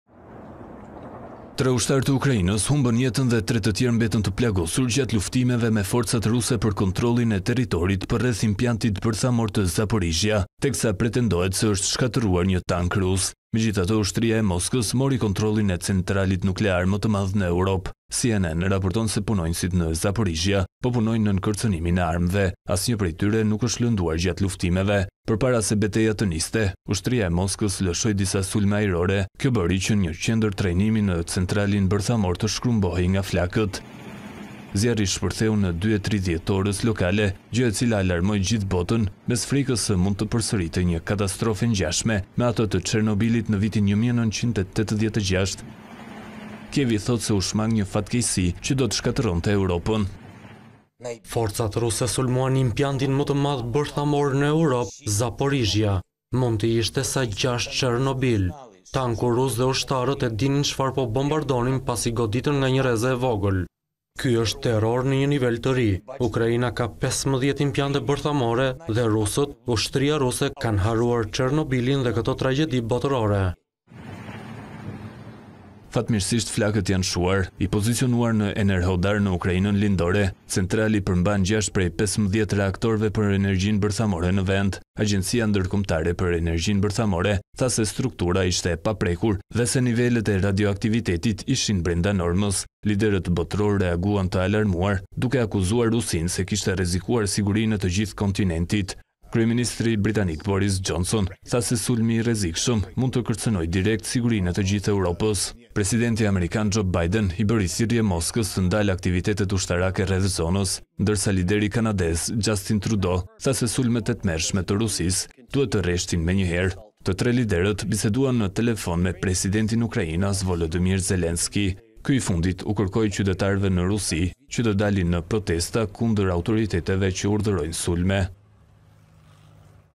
Punonjësit me armë në kokë, sulmi në impiantin bërthamor në Ukrainë
Tre ushtarë të Ukrainës humbën jetën dhe 3 të tjerë mbetën të plagosur gjatë luftimeve me forcat ruse për kontrollin e territorit përreth impiantit bërthamor të Zaporizhzia.